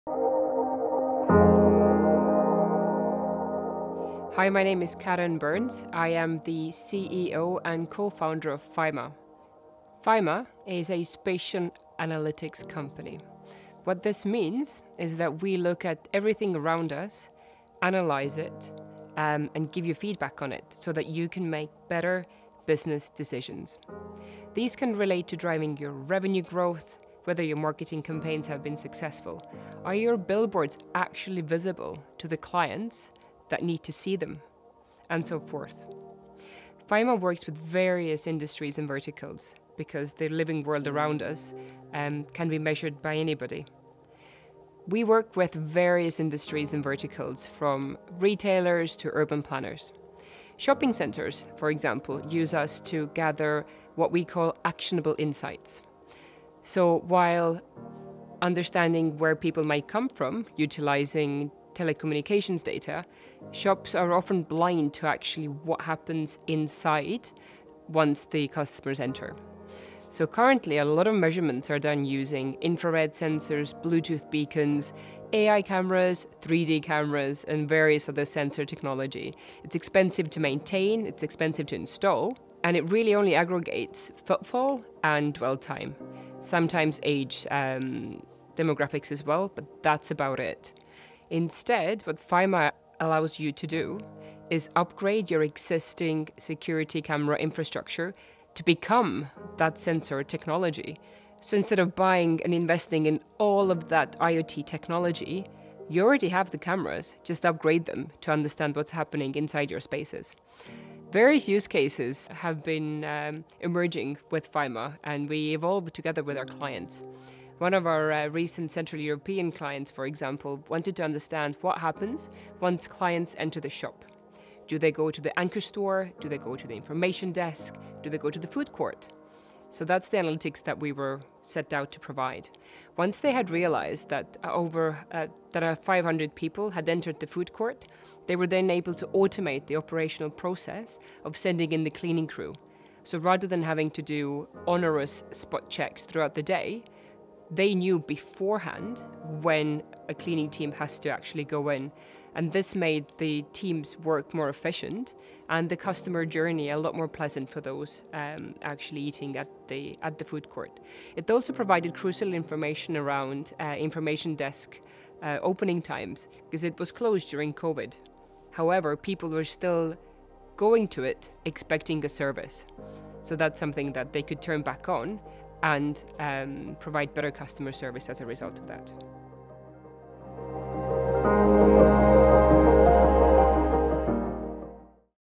FYMA | Turn your existing camera setup into world-class data collection and analysis tool
Karen K. Burns, Co-Founder, and CEO at Fyma introduces what is Fyma (For-Your-Motion-Analytics) and what you can achieve with a computer vision AI-powered platform that allows you to enhance your existing camera equipment into a powerful analytics tool.
By leveraging existing video footage, urban planners and business owners can understand activity and behavior in their spaces to make more granular and informed business decisions. No additional hardware needed. 100% GDPR compliant.

✔ In-store customer analytics.
✔ Real-time data for urban planning and traffic management.
✔ Understand the effectiveness of targeted billboard advertising.

✅ Privacy Guaranteed.
Fyma is 100% compliant with existing privacy policies including GDPR.

✅ No additional hardware is needed. ​
No setup fee. No hardware fee. No license fee. Only actionable data from your video stream.

✅ Get actionable data insight.
The Fyma solution is a self-learning application. This removes the burden of manually inputting hundreds or thousands of business rules or variables and significantly reduces setup time.

TIMESTAMPS:
00:00 Intro
00:10 What is Fyma - a computer vision analytics tool?
00:50 Actionable instore-analytics for shopping centers.
01:10 What current solutions are missing?
01:31 Actionable data from your existing video stream.
01:57 Case study with a shopping center.